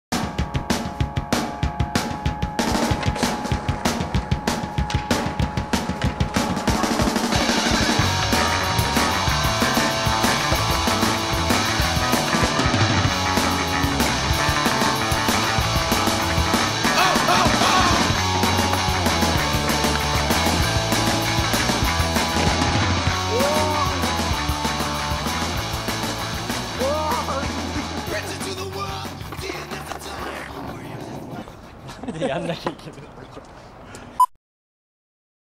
HARD zeiss yukihisa nakamura AD
41VM関西支部局の田中竜一が盗撮＆編集した中村晋久の動画ＣＭです。ギコイというかヘタすぎ！もうちょっとスケートします。すいません・・・。